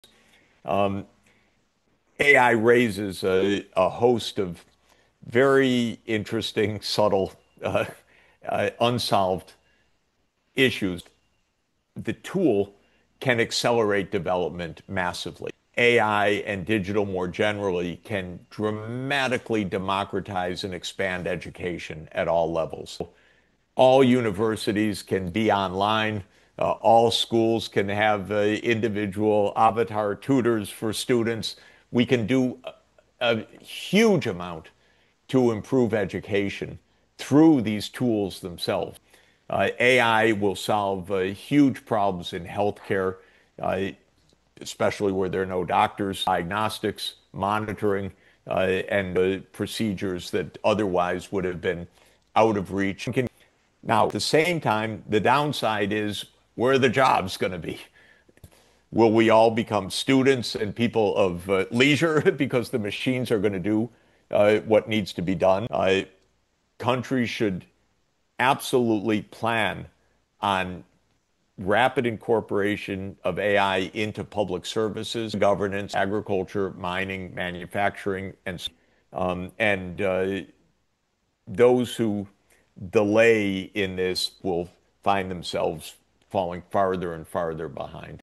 Will AI be our greatest ally or the downfall of education? Discover the truth!
Exploring the transformative power of artificial intelligence in education and global development, this thought-provoking video dives deep into the dichotomy of AI's potential and its pitfalls. From the hopeful vision of personalized education with avatar tutors to pressing concerns about job displacement and trust erosion in classrooms, the insights shared by Jeffrey Sachs and Norman Finkelstein set the stage for a fiery debate. As AI reshapes industries from healthcare to governance, the need for strategic planning becomes paramount. What does the future hold for students and educators in this new age of technology? Buckle up as this conversation unravels the complexities of our rapidly evolving world!